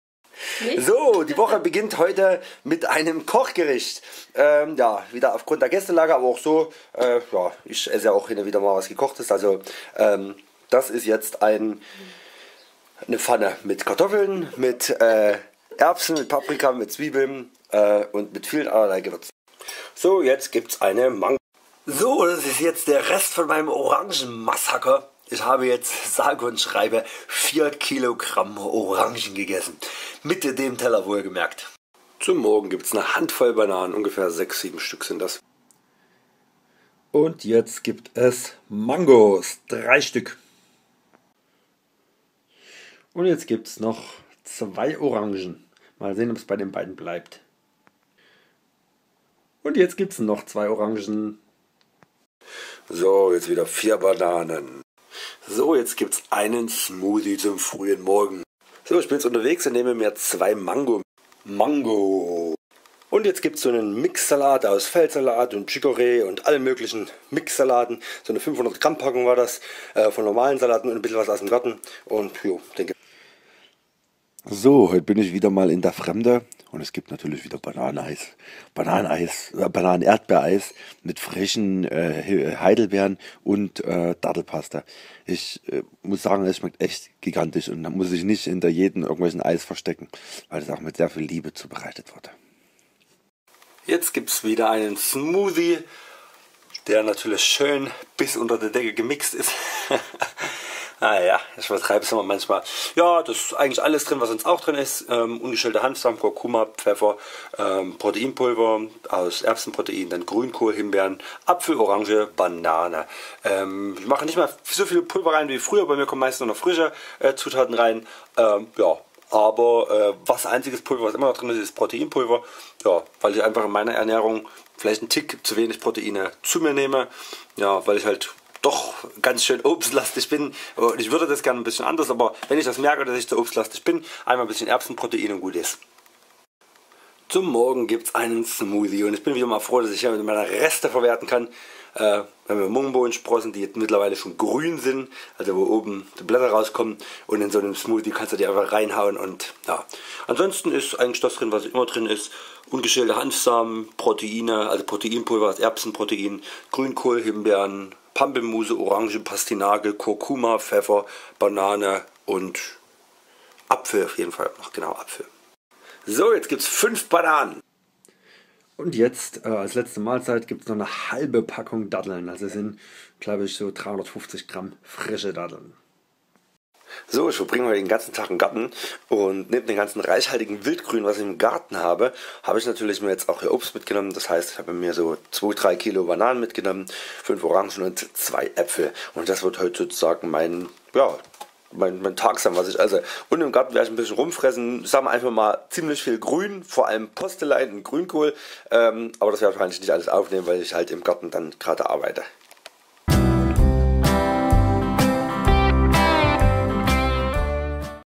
Rohgan Food Diary & Vegane Rohkost als Inspiration für eine gesunde Ernährung
Heute wieder mein veganes Food Diary mit Rohkost. Gesunde Inspirationen für Rohganer, Rohköstler, Veganer,Vegetarier und die die es werden wollen. Abnehmen, Durchstarten, Wohlfühlen mit einfacher und gesunder Ernährung!

Wenn ihr mir einen Brief schreiben wollt, ist meine Adresse:

Christian Pörtzel
Friedrich Engels Strasse 55
04425 Taucha